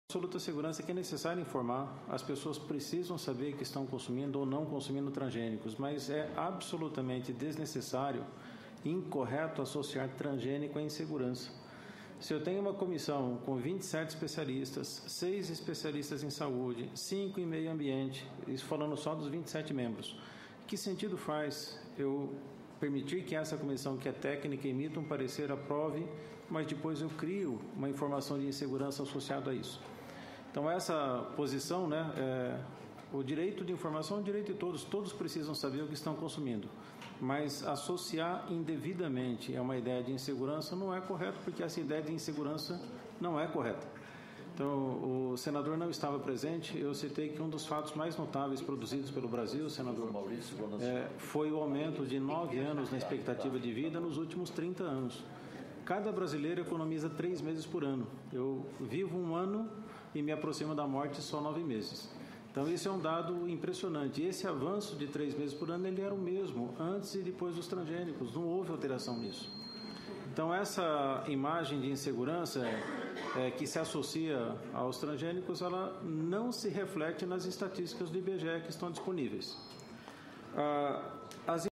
Apenas 6% dos brasileiros entendem rótulo de transgênico nas embalagens (3/3)
8/11/2015 - CCT/CMA - Apenas 6% dos brasileiros entendem rótulo de transgênico nas embalagens (3/3)